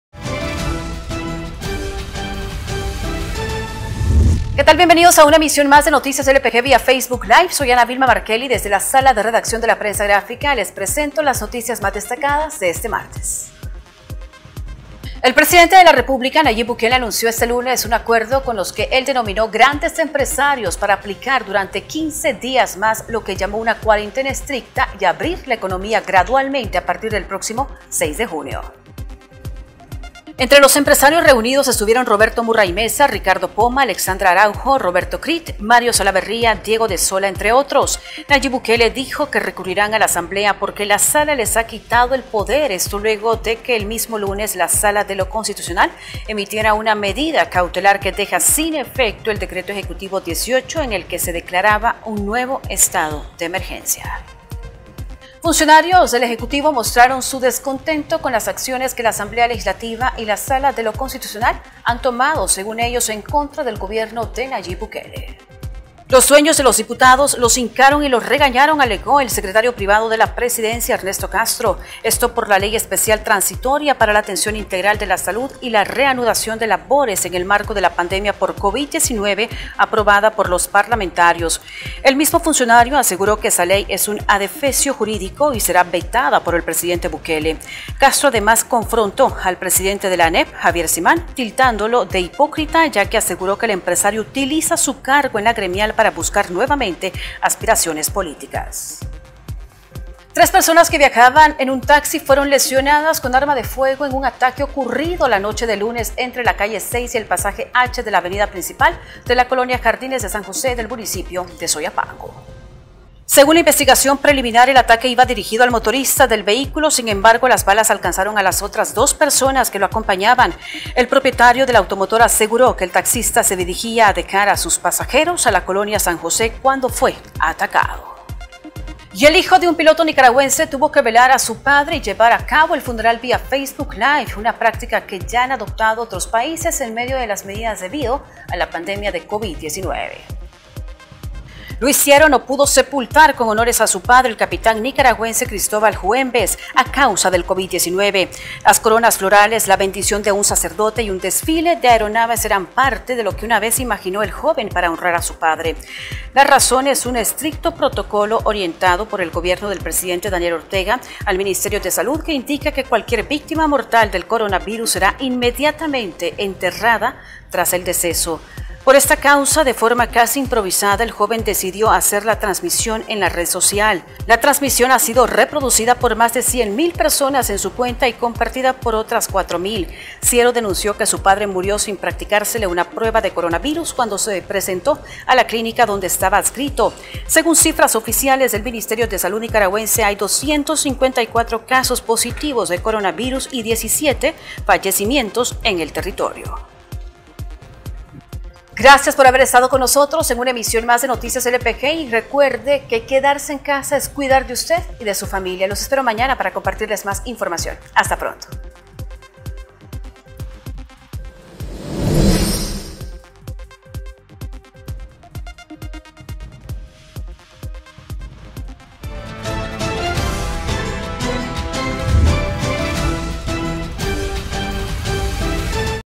Noticiero LPG 19 de mayo: "Los hincaron y los regañaron" dice secretario de Bukele a diputados
Esto por la "Ley Especial Transitoria para la Atención Integral de la Salud y la Reanudación de Labores en el Marco de la Pandemia por covid-19", aprobada por los parlamentarios.

¡Visítenos!